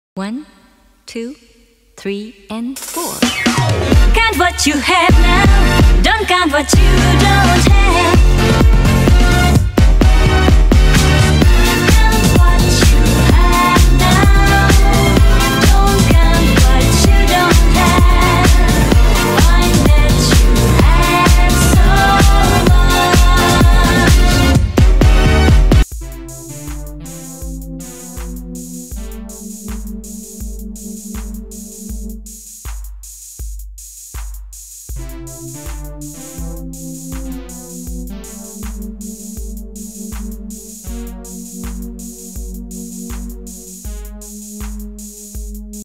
Ludwig Bangers
Ludwig shows of his skills with a banger.